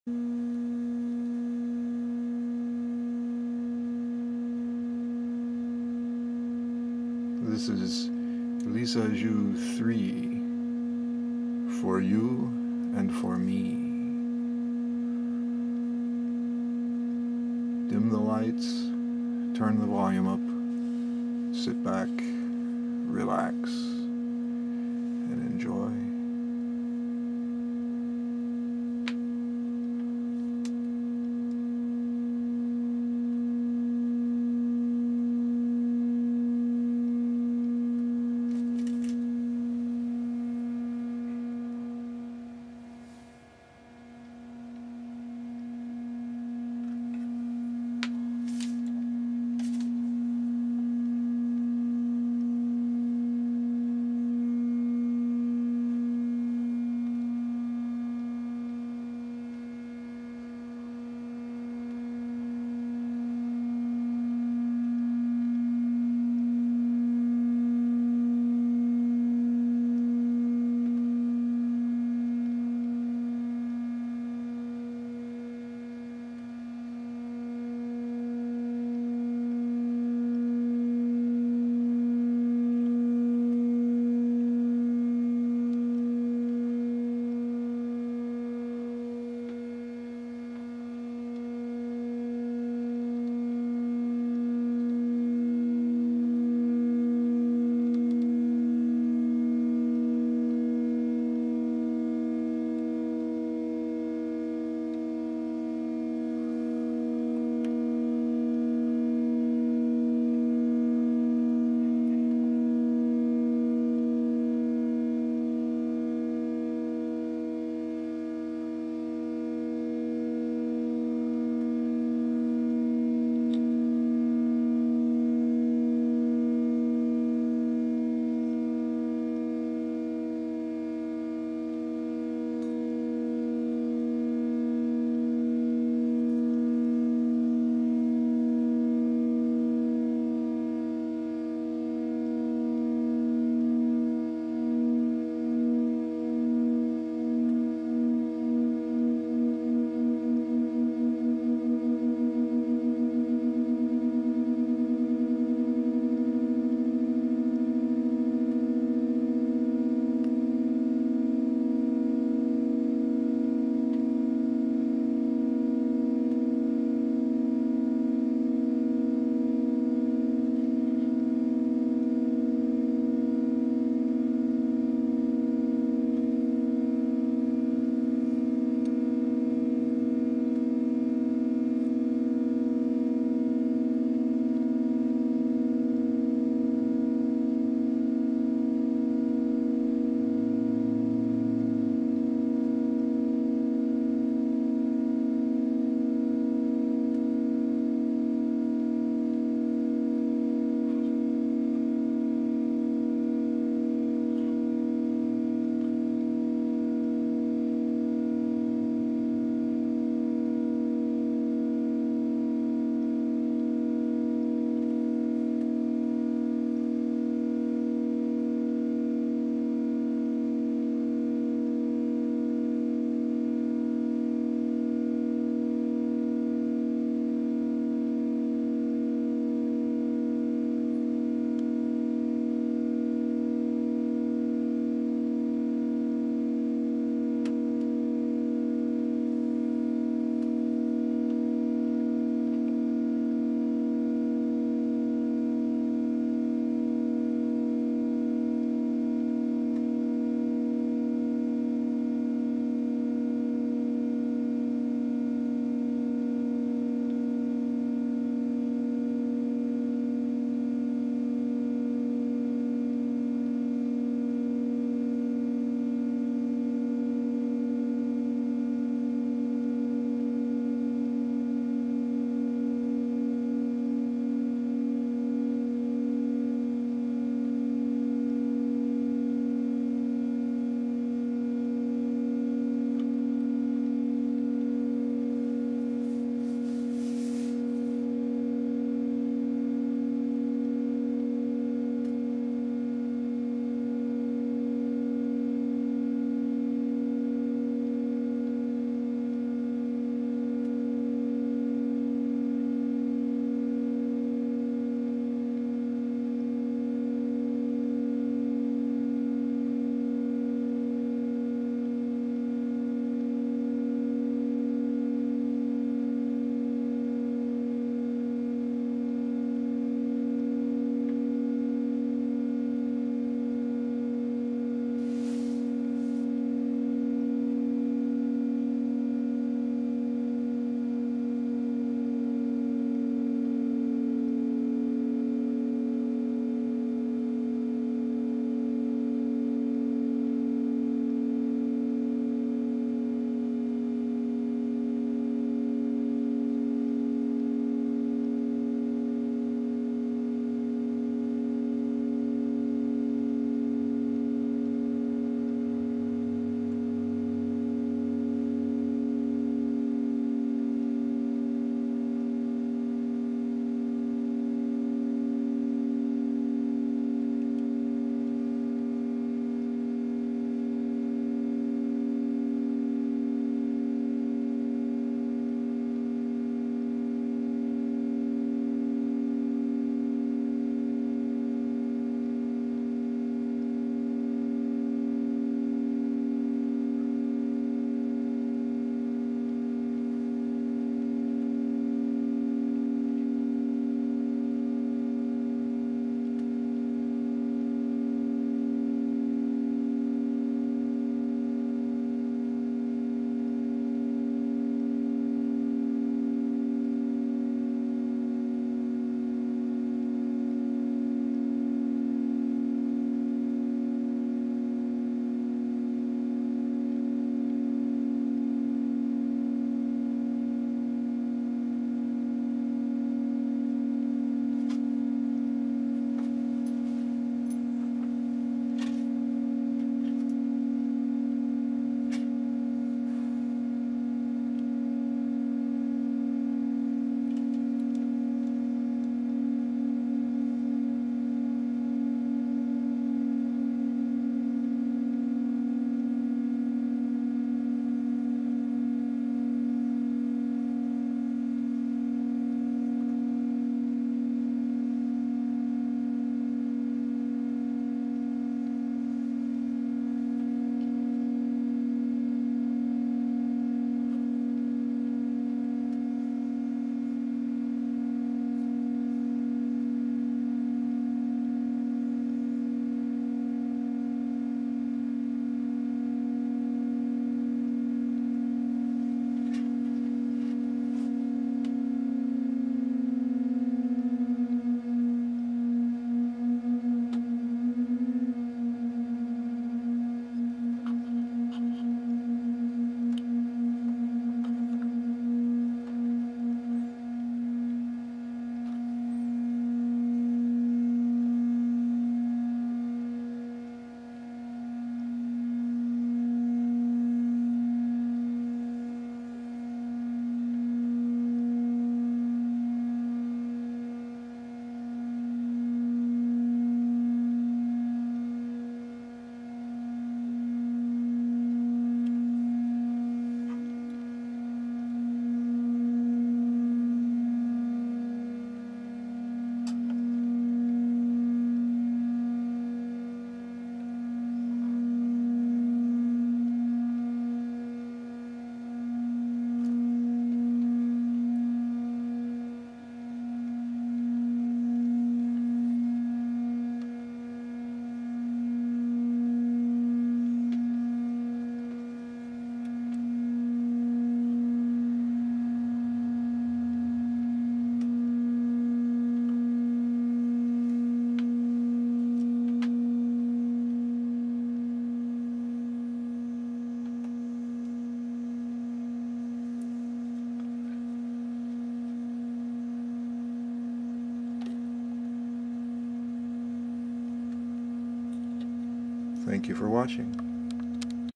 RM503 and the 4 Oscillators Present: Lissajous For You and Me: Part 3
More explorations of the 4 oscillators Lissajous space.

I am beginning to think that all shapes and motions are capable of being expressed by combinations of interacting sine waves in different dimensions.

I don't know whether this is a profound insight or just a fast Fourier transform. Or both.

Whatever it is, I'm pretty sure there's a sine wave combination that looks like it.

Equipment:
Tektronix RM503 precision low-frequency oscilloscope
Dumont Type 347 Oscillator
General Radio 1310B Oscillator
WaveTek Model III Voltage Controlled Oscillator
Interstate F43 High Voltage Function Generator